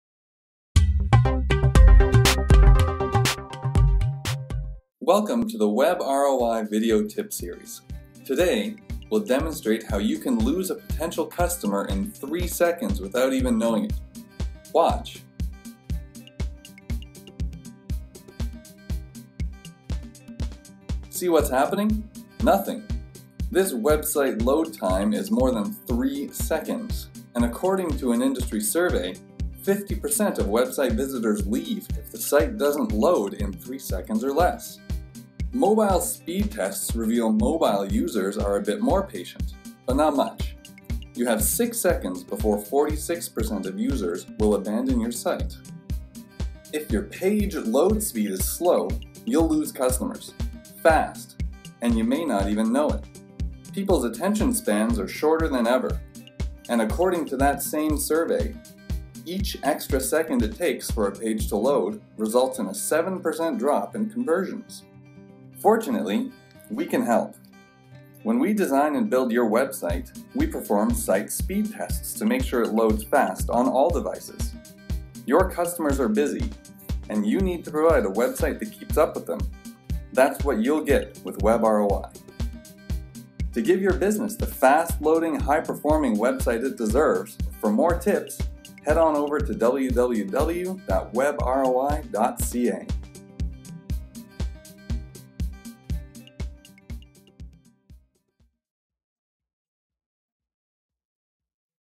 WEB ROI Video Tip: Avoid Losing Customers in 3 Seconds
3 seconds is all you get for your website to properly & fully load. Watch this short video to find how the impact on your business when your site takes longer than that.